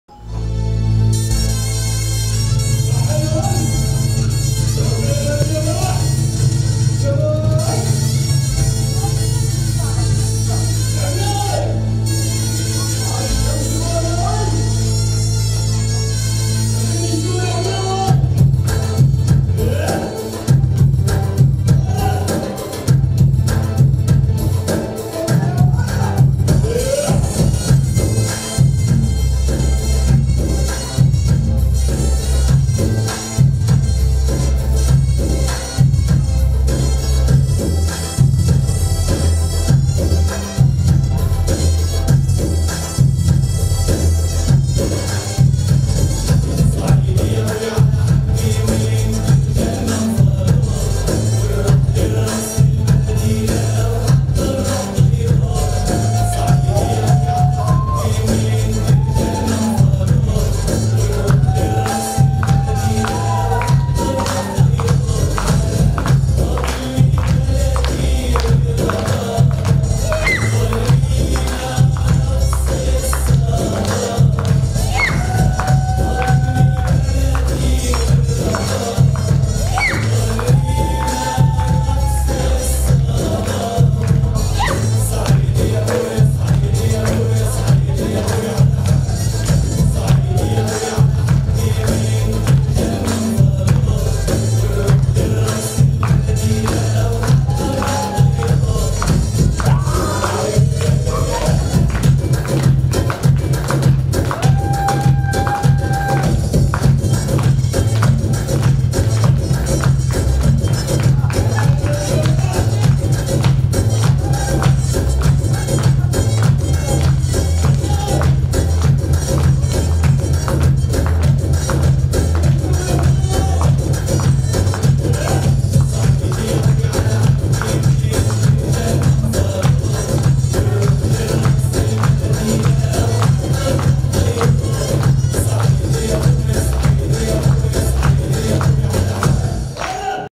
ГРУППОВАЯ ИМПРОВИЗАЦИЯ БАТТЛ Согдиана дети & Согдиана Boys младшие
Фестиваль «СОГДИАНА ЗАЖИГАЕТ ЗВЕЗДЫ 2021»
Сосногорск 2021
Дом детского творчества